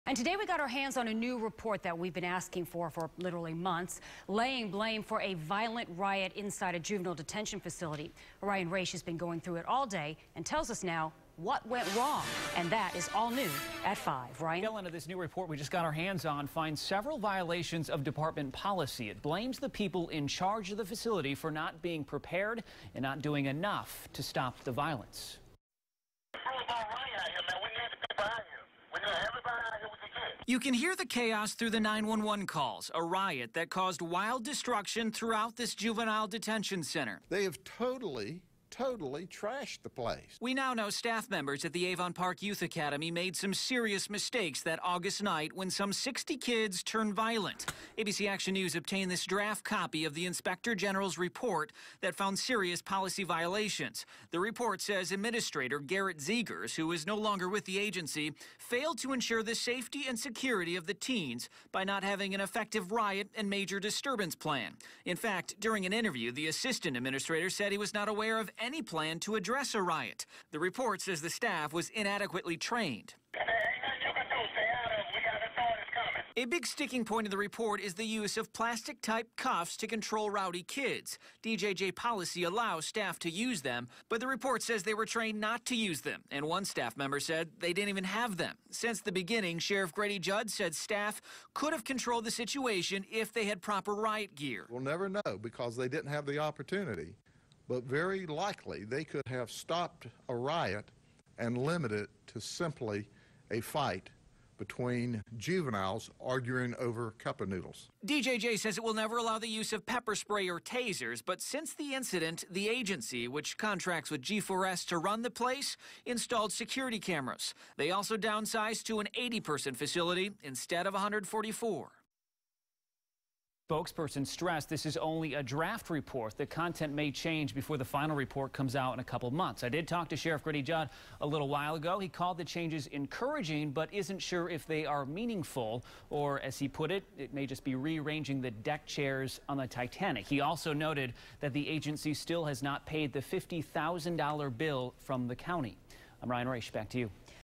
Florida Department of Juvenile Justice releases report on Avon Park facility riot
A Florida Department of Juvenile Justice report on a so-called riot at its Avon Park facility finds violations.